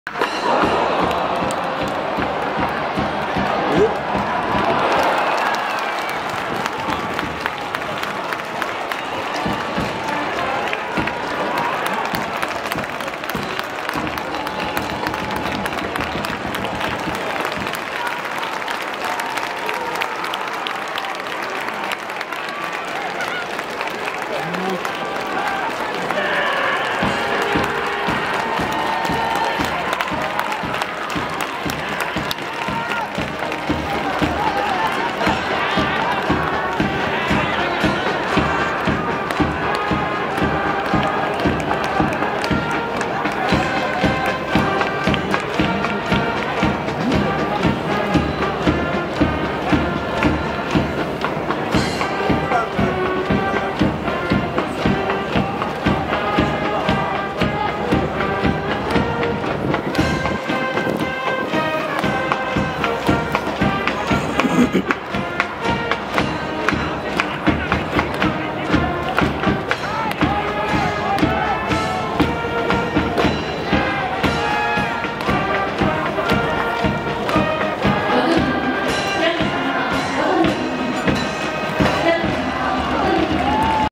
青山学院大学、中田達也満塁ホームラン
2024/11/25
青山学院大学対創価大学
青山学院大学、中田達也、田代から
満塁ホームラン。
ちょっと疑惑の判定に。